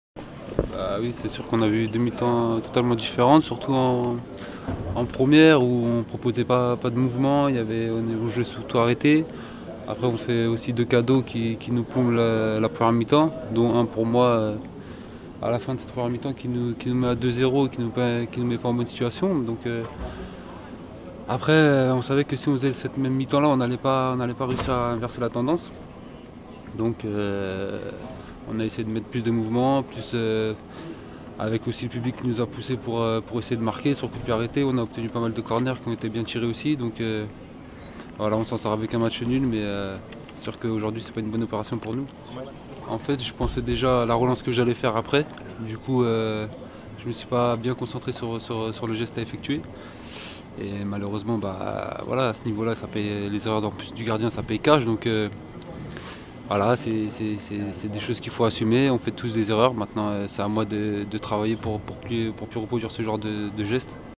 OM-Rennes : Pelé explique les raisons de sa faute de main
Yohann Pelé a assumé son erreur en zone mixte après le match nul 2-2 des Olympiens contre Rennes, ce dimanche soir. Auteur d'une faute de main sur le deuxième but des Bretons, le portier olympien en a expliqué la raison."J'étais déjà concentré sur le relance que j'allais faire. Du coup, je ne me suis pas concentré sur le geste à effectuer et malheureusement les erreurs à ce niveau-là on les paye cash. Il faut assumer, on fait tous des erreurs et c'est à moi de travailler pour ne plus reproduire ce genre de geste", a-t-il déclaré.